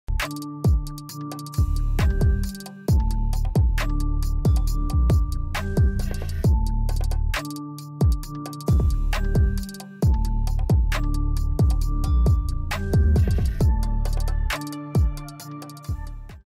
Single Arm Bent Over Cable Reverse Fly
Start the Single Arm Bent Over Cable Reverse Fly standing side on to the cable machine hinged at the hips. Then grab the cable with your outside hand. Keeping your arm locked out pull the cable away from you until your arm is fully extended away from you.